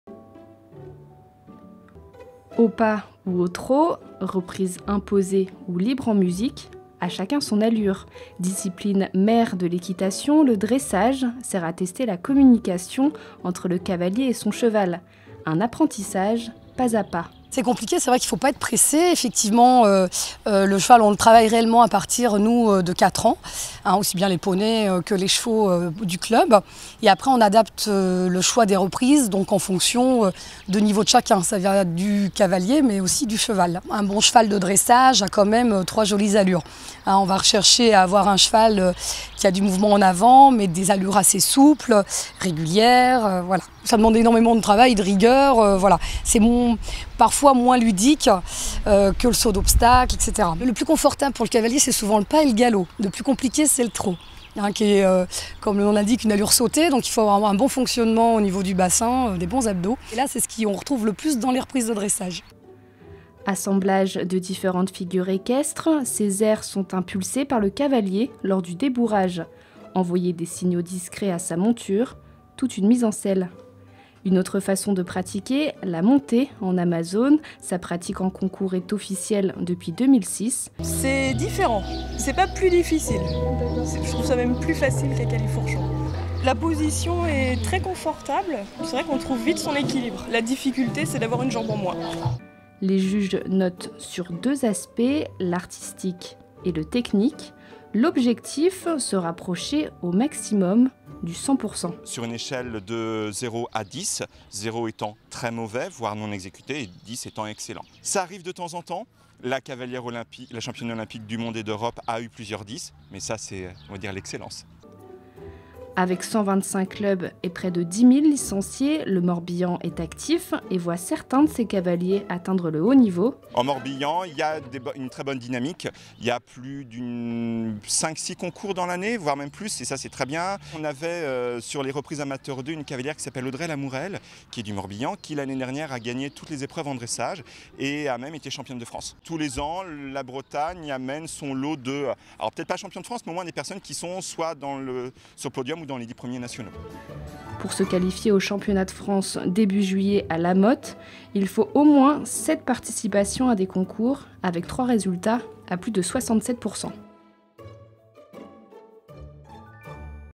Dressage équestre : l'art de la fusion
video - Discipline mère de l'équitation, le dressage est la communion entre le cavalier et sa monture. L'apprentissage difficile, se fait pas à pas. Comme en témoigne ce reportage de Clara Jestin, pour la rédaction des sports de TébéSud, aux écuries du Vieux Puits à Questembert.